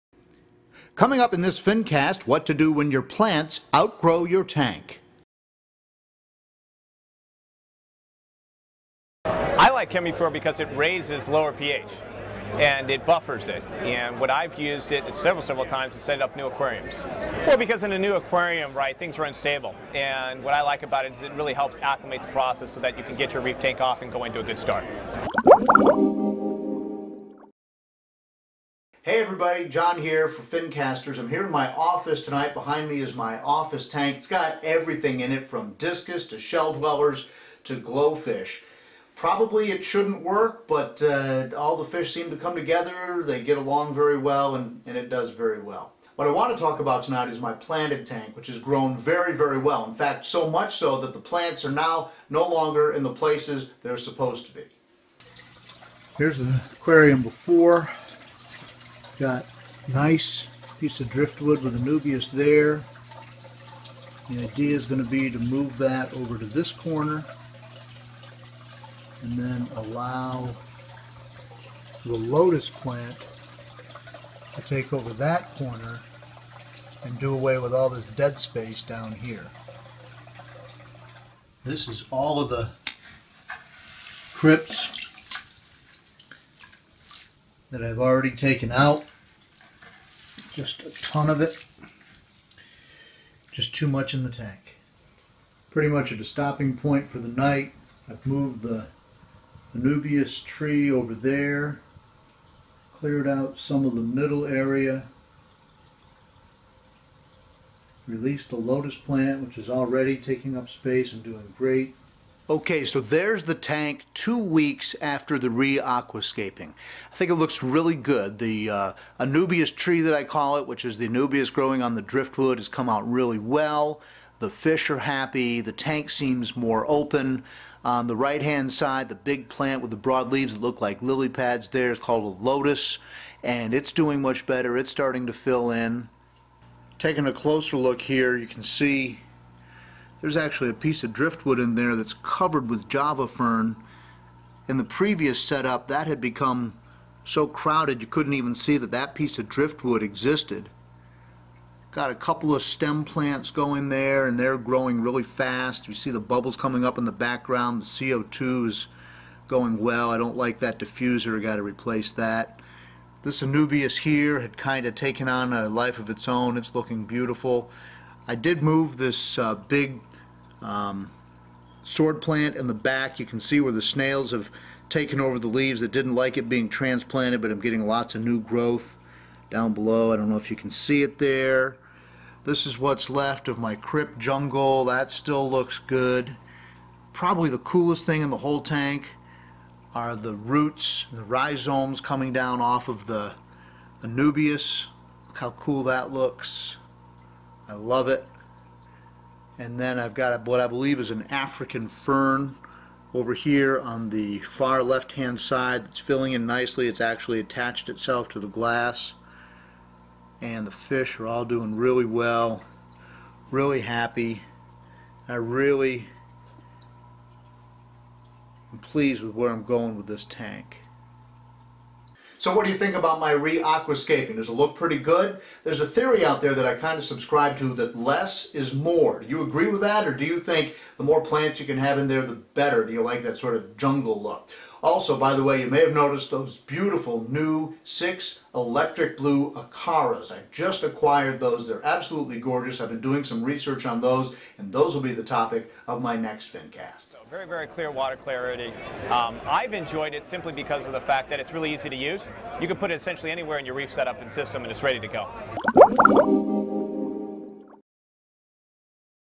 Fincasters Eposode 28 Planted Tank Do Over Aquascaping a Planted Aquarium
John's 55 gallon planted tank is a victim of it's own success.  The original aqua-scape had become over grown and completely lost it's sense of balance.  John rolled up his sleeves and transformed the aquarium back to something beautiful.